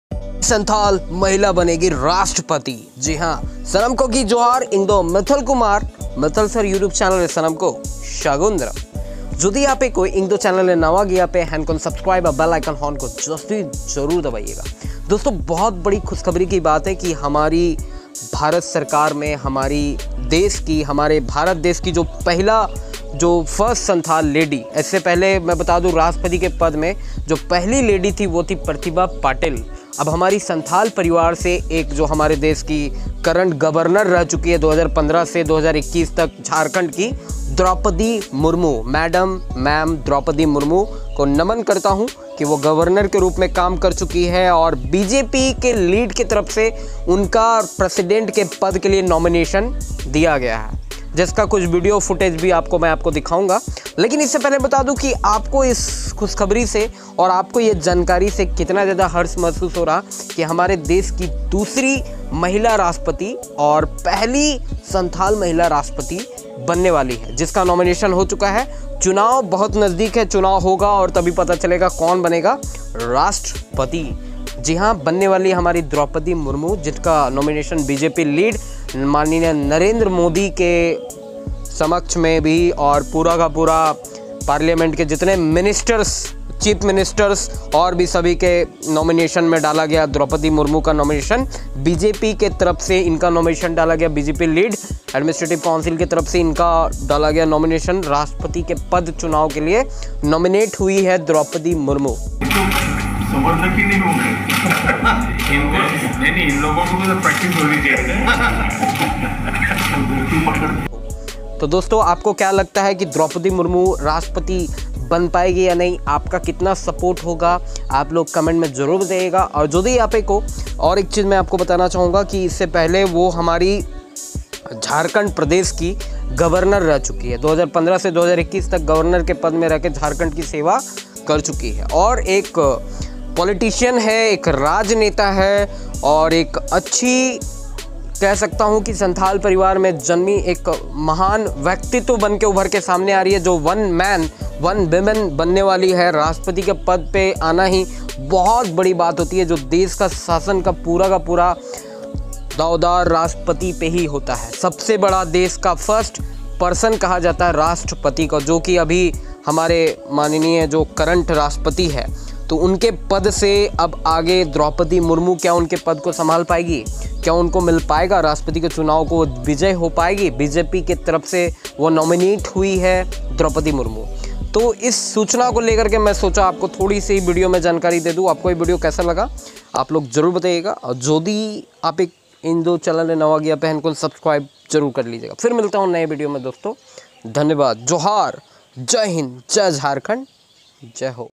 India's First Santhal Woman President || Draupadi Murmu
Johar 🙏
Draupadi Murmu is an Indian politician. She is a member of the Bharatiya Janata Party. She is the official nominee of the BJP-led National Democratic Alliance for the 2022 presidential election. She previously served as the ninth Governor of Jharkhand from 2015 to 2021